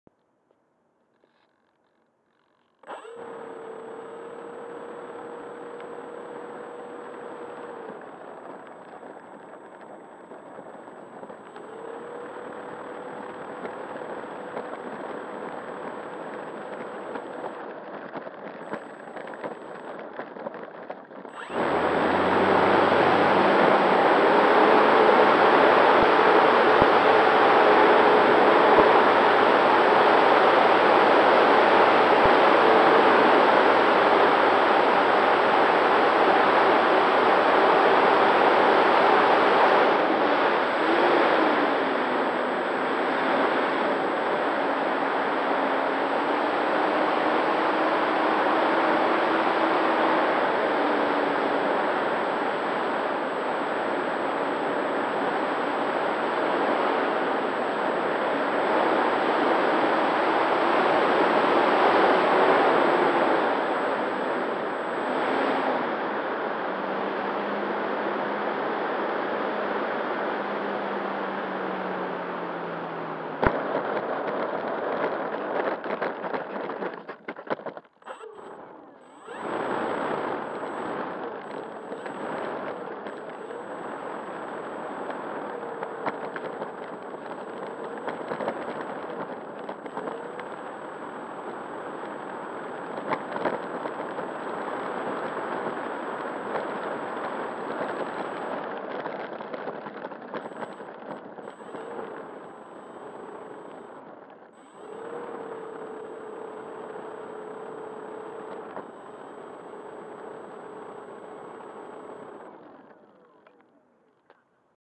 Attempted A Flight In Dense Morning Fog With My P-51D Mustang 10/07/2020 RunCam2 Mounted For FPV
The fog was super dense this morning. I tried to get a flight to see what it was like. NOPE! Plane disappeared into the fog right after take off. Thank GOD for SAFE!